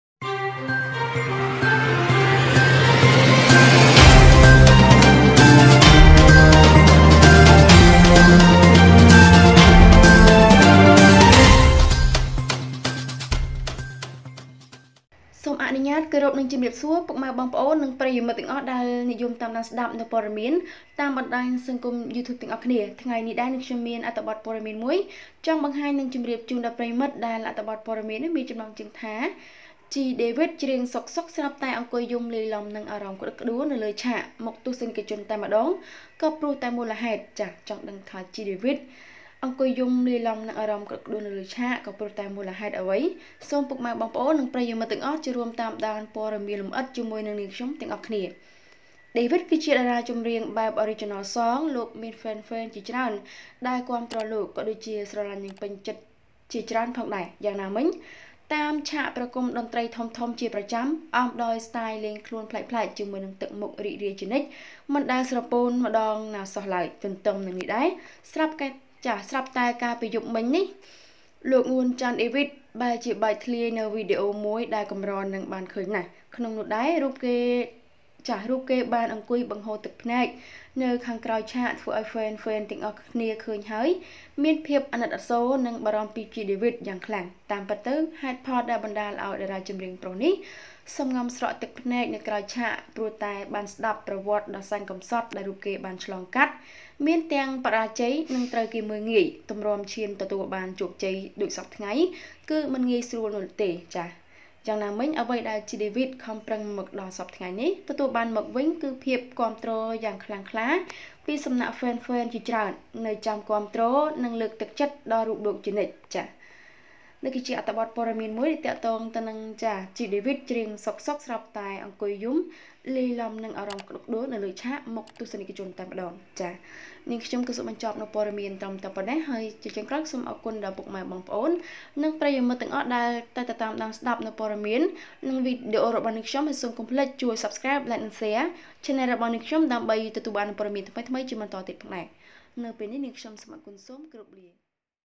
ជី ដេវីត ច្រៀងៗសុខៗស្រាប់តែអង្គុយយំលាយឡំនិងអារម្មណ៌ក្តុកក្តួលនៅលើឆាក មុខទស្សនិជនតែម្តងក៌ព្រោះតែមូលហេ
Giving David a good chuckle, he sat down and wept on the stage for the audience.

Thanks!